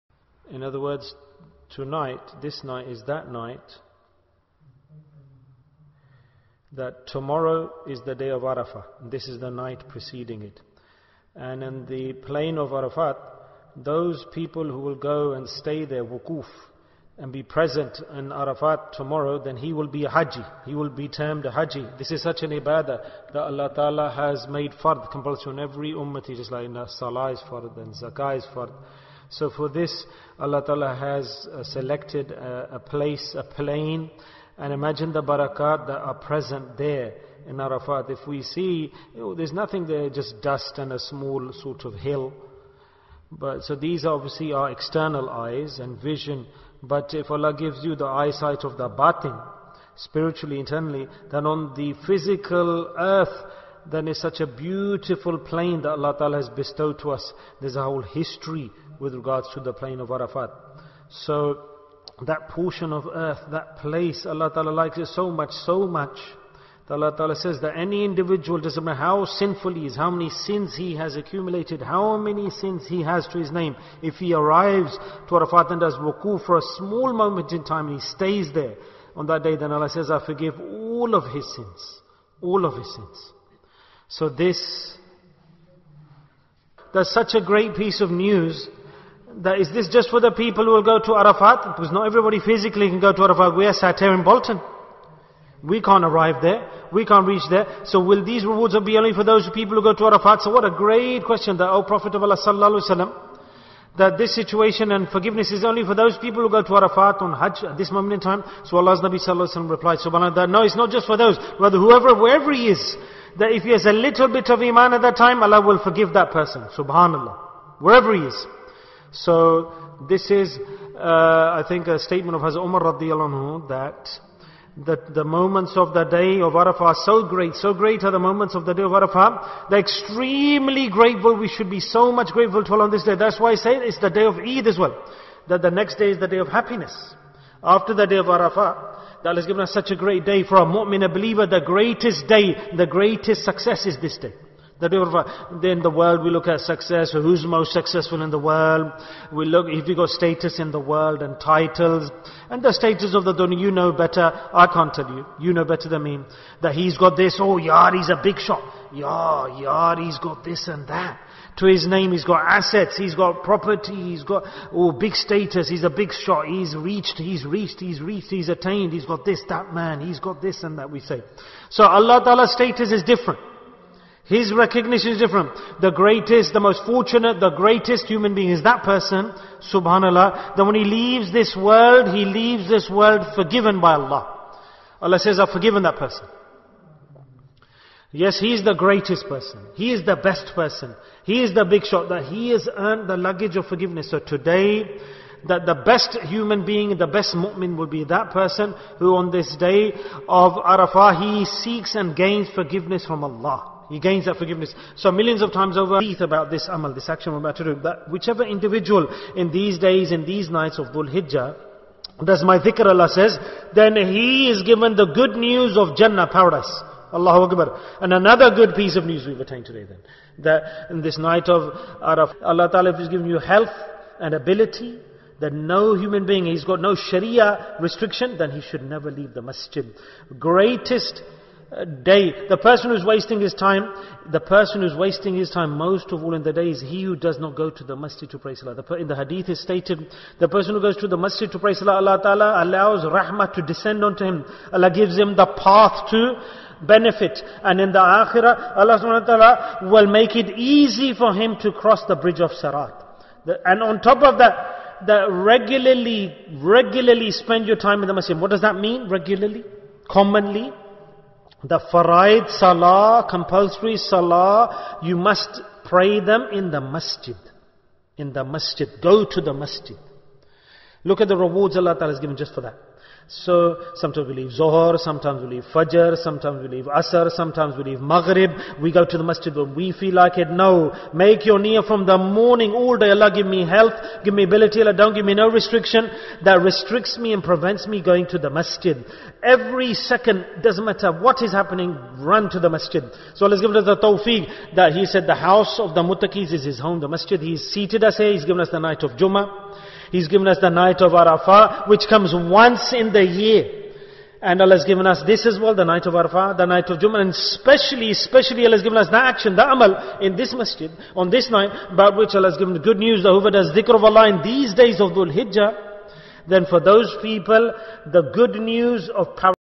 Clips Dhikr in the Night of Arafah - by Hadhrat Sheikh Asif Hussain Farooqui Naqshbandi (UK)
Clips by Hadhrat Sheikh Asif Hussain Farooqui Daamat Barakaatuhum (UK). for Daily Live Dhikr. Follow Dhikr Live on Facebook, Instagram & Twitter